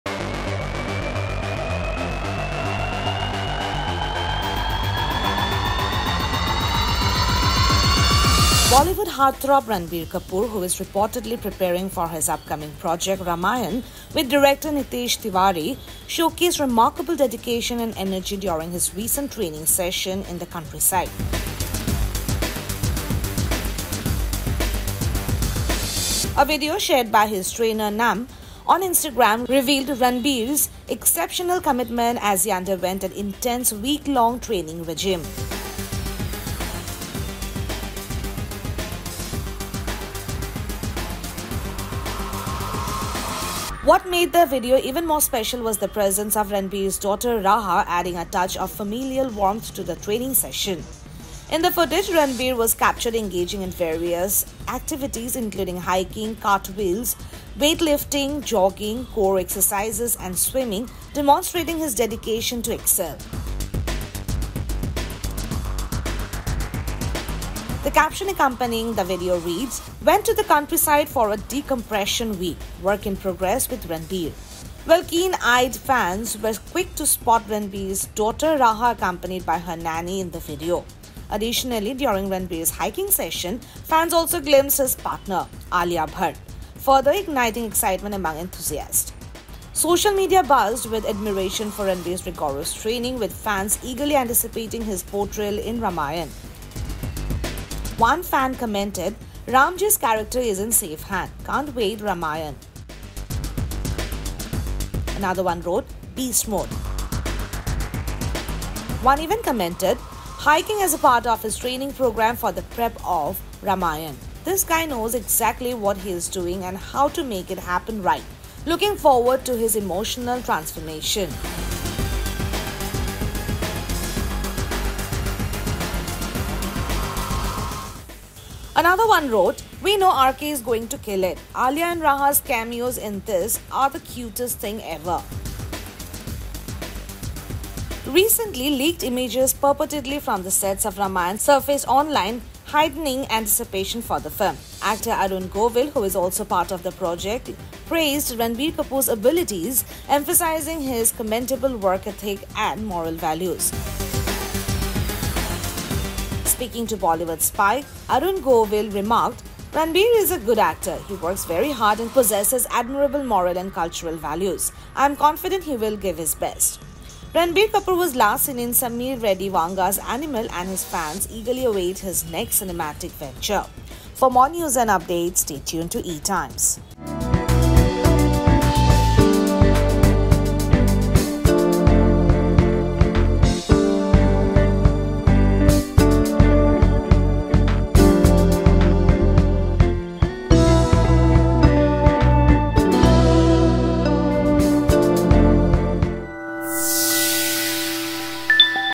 Fans spot Alia Bhatt & Raha in this workout video of Ranbir Kapoor preparing for 'Ramayana'
Bollywood heartthrob Ranbir Kapoor, who is reportedly preparing for his upcoming project "Ramayana" with director Nitish Tiwari, showcased remarkable dedication and energy during his recent training session in the countryside. A video shared by his trainer, Nam, on Instagram revealed Ranbir's exceptional commitment as he underwent an intense week-long training regimen. What made the video even more special was the presence of Ranbir's daughter, Raha, adding a touch of familial warmth to the training session.